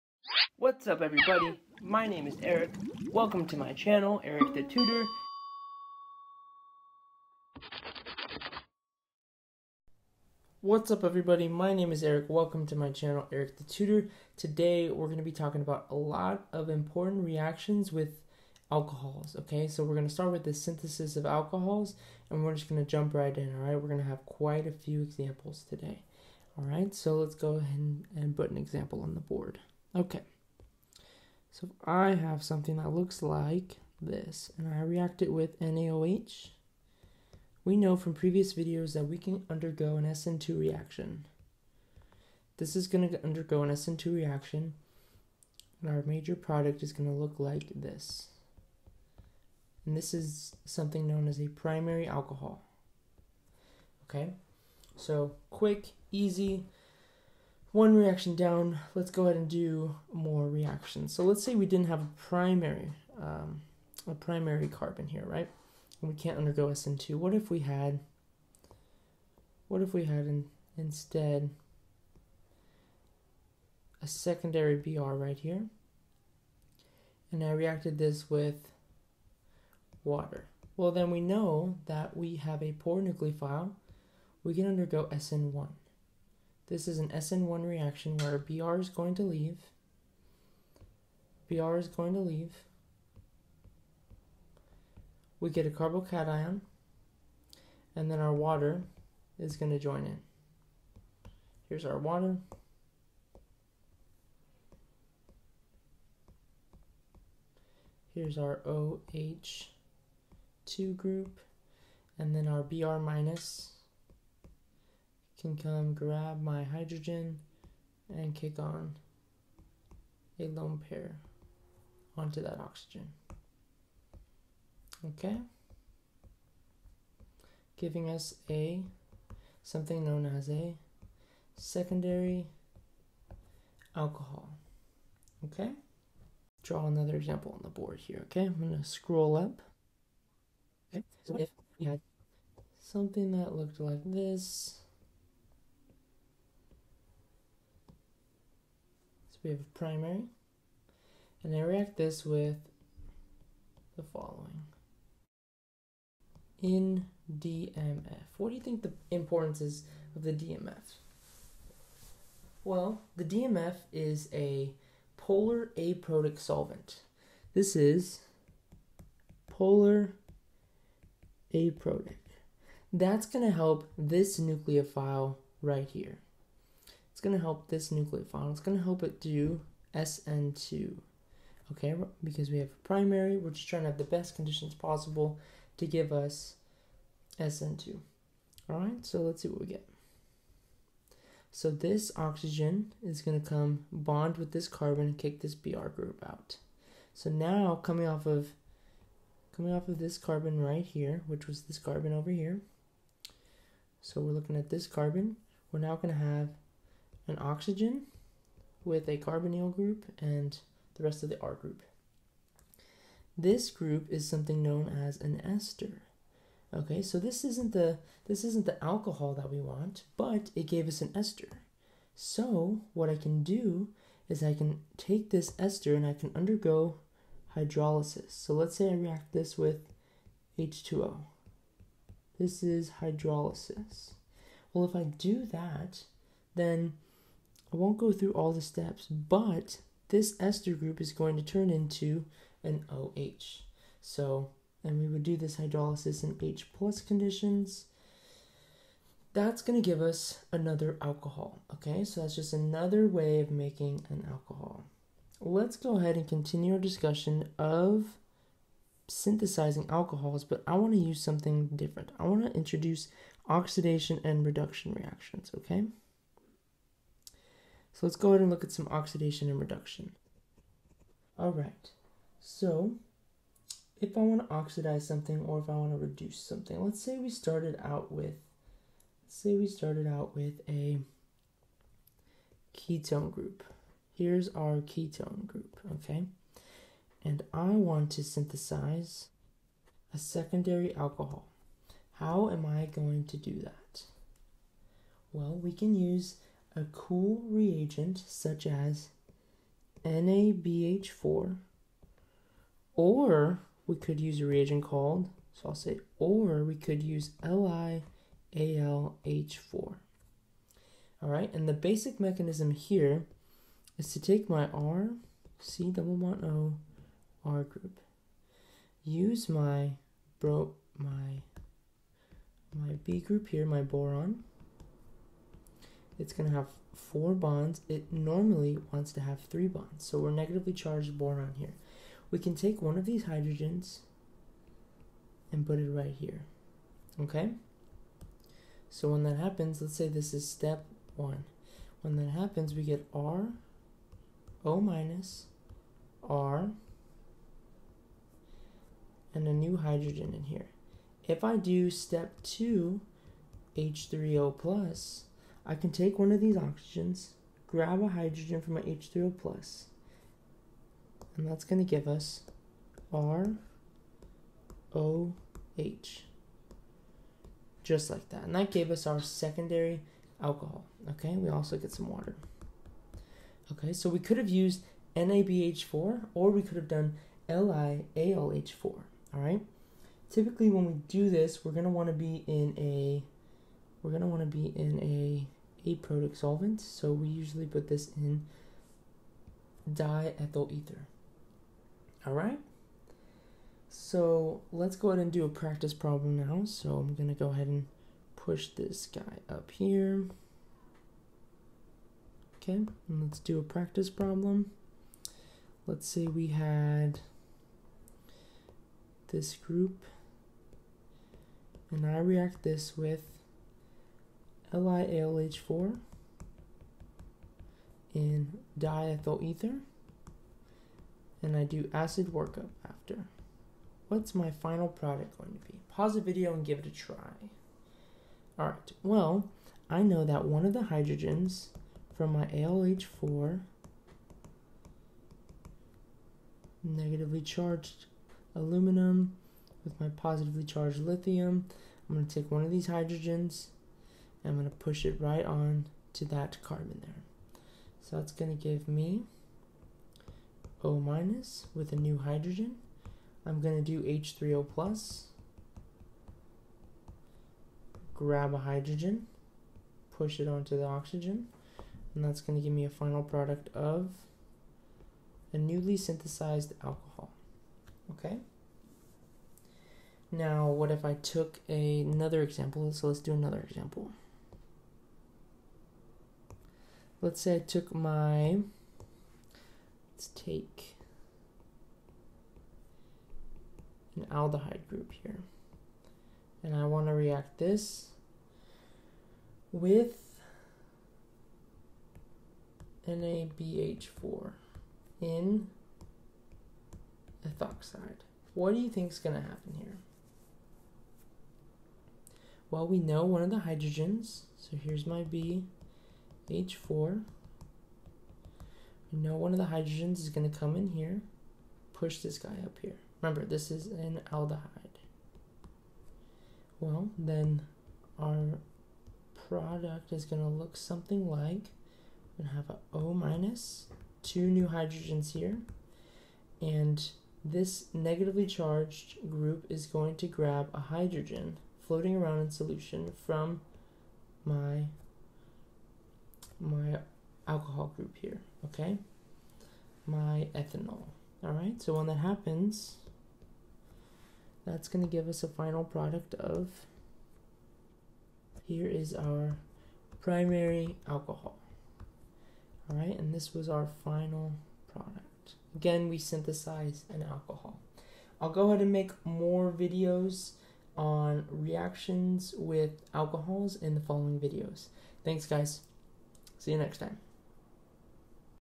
Synthesis of Alcohols | Organic Chemistry | UC Davis | Eric The Tutor
In today’s video, we continue our series on alchols by discussing the synthesis of alcohols. The reactions we examine today: -SN2 and SN1 formation of alcohols, ester hydrolysis to form alcohols, reductions of ketones to secondary alcohols via LiAlH4 or NaBH4, and reductions of aldehydes to primary alcohols via NaBH4. Enjoy!